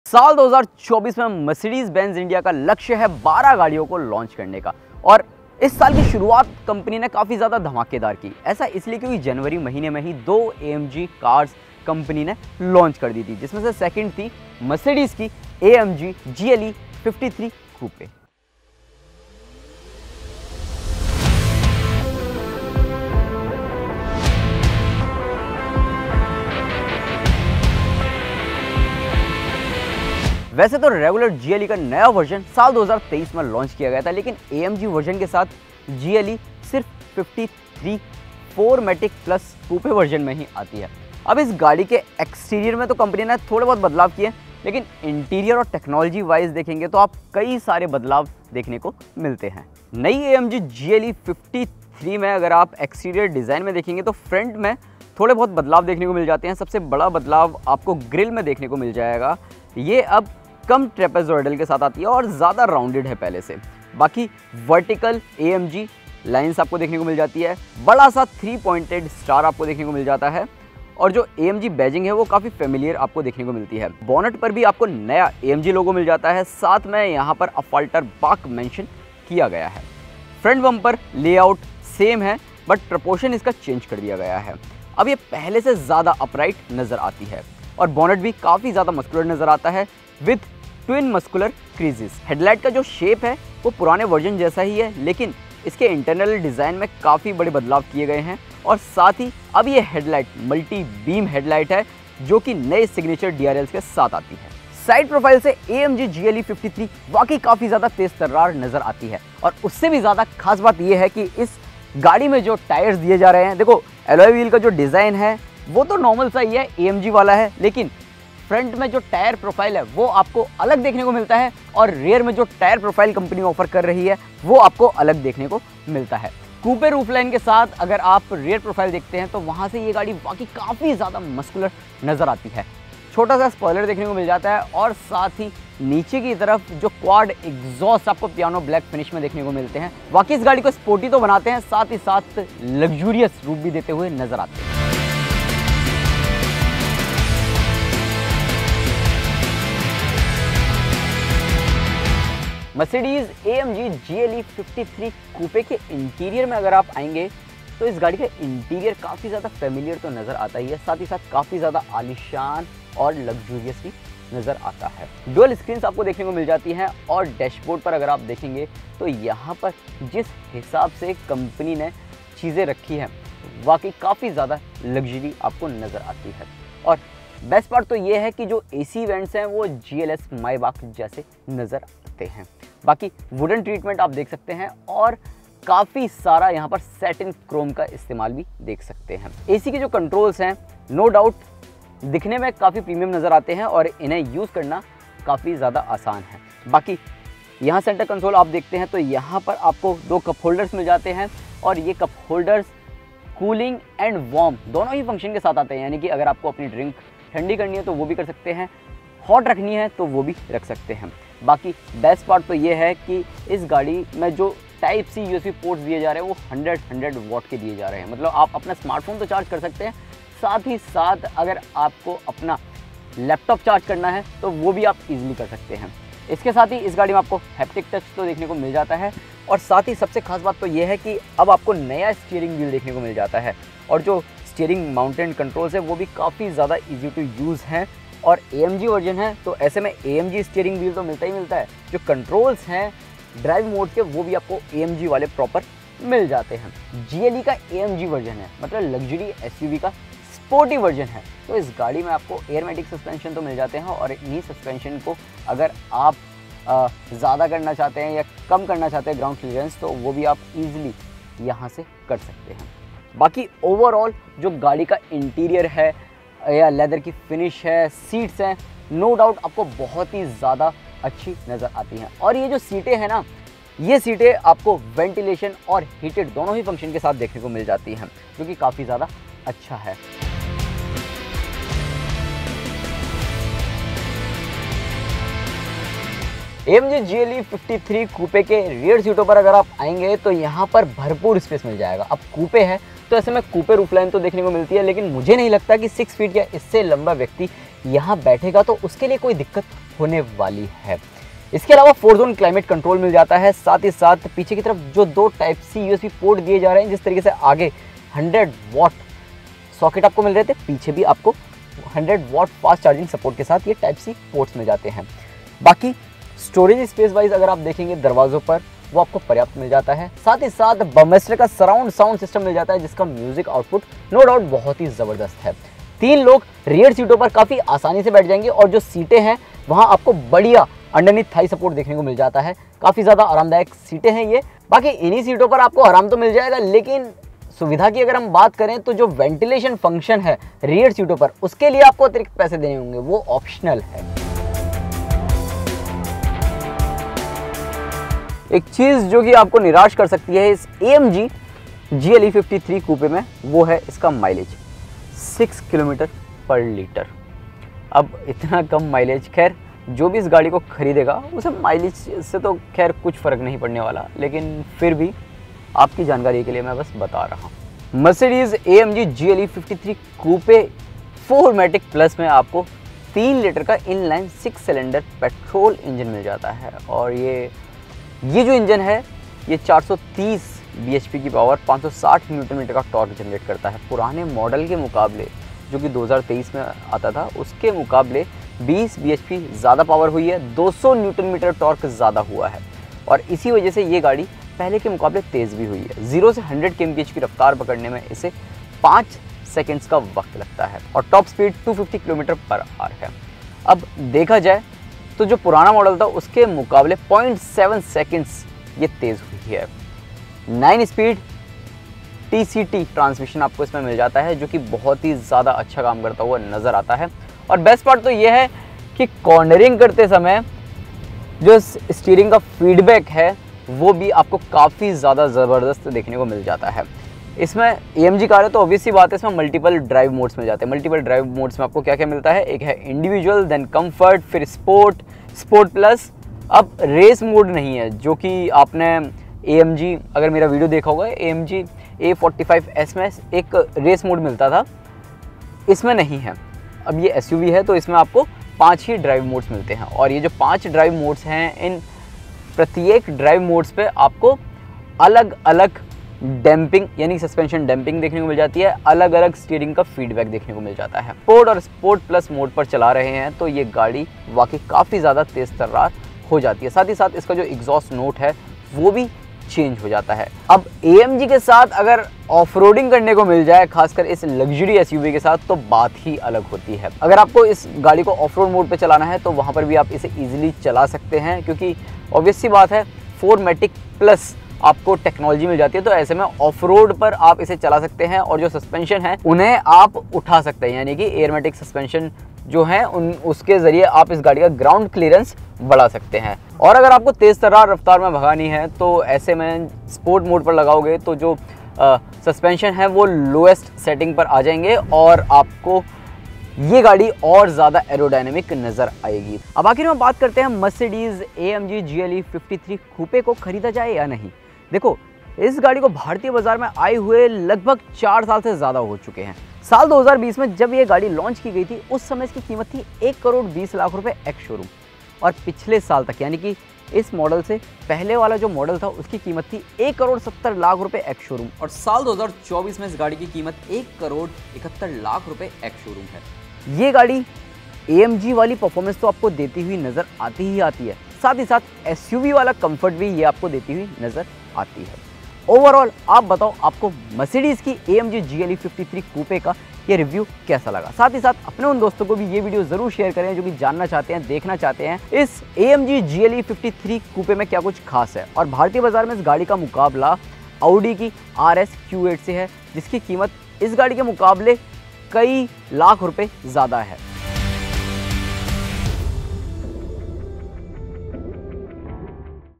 2024 Mercedes-AMG GLE 53 Coupe Review | The Ultimate Sports SUV!
Today, we're diving into the exhilarating world of the 2024 Mercedes-AMG GLE 53 Coupe, a true marvel of engineering that blends luxury, power, and cutting-edge technology. Whether you're a car enthusiast or in the market for a high-performance SUV, this review is a must-watch!